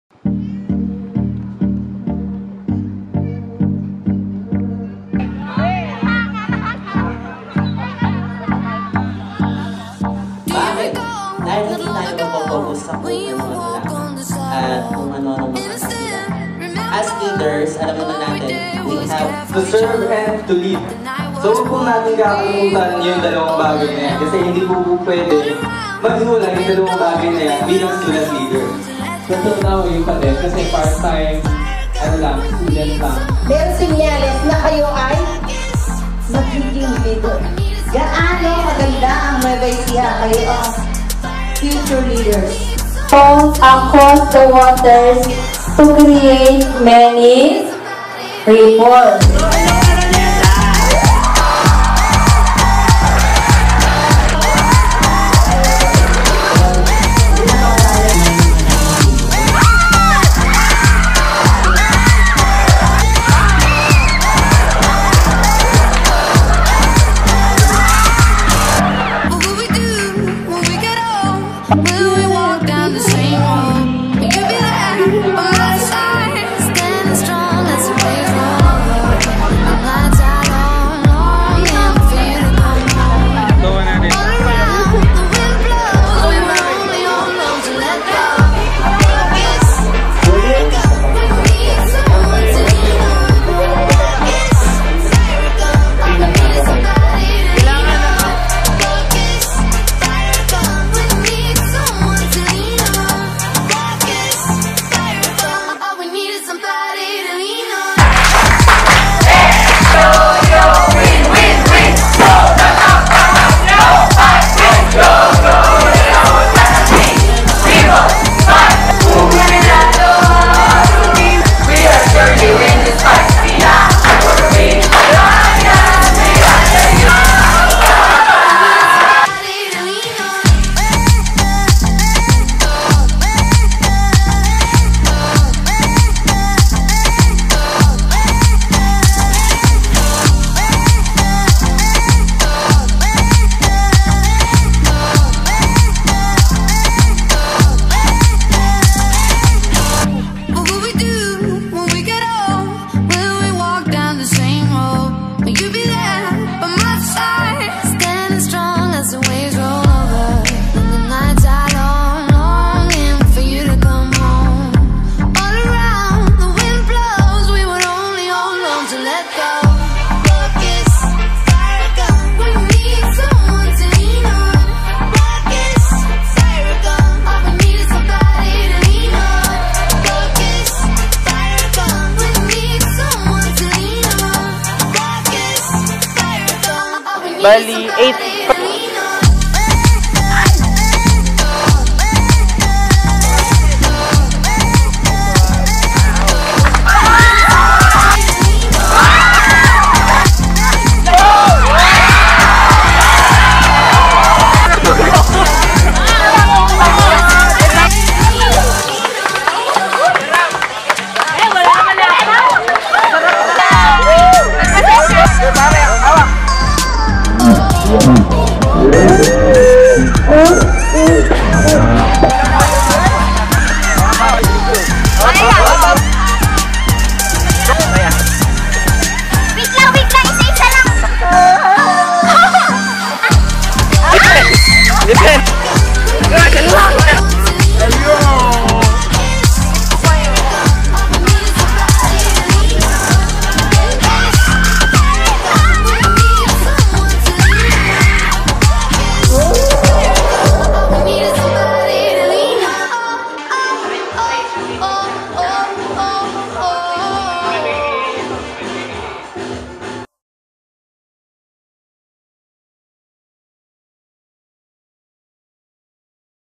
NEUST- 19th Annual Leadership Training Seminar Teaser 2
Held at Hunter Valley Resort, Cabu Cabanatuan City from July 8-10, 2016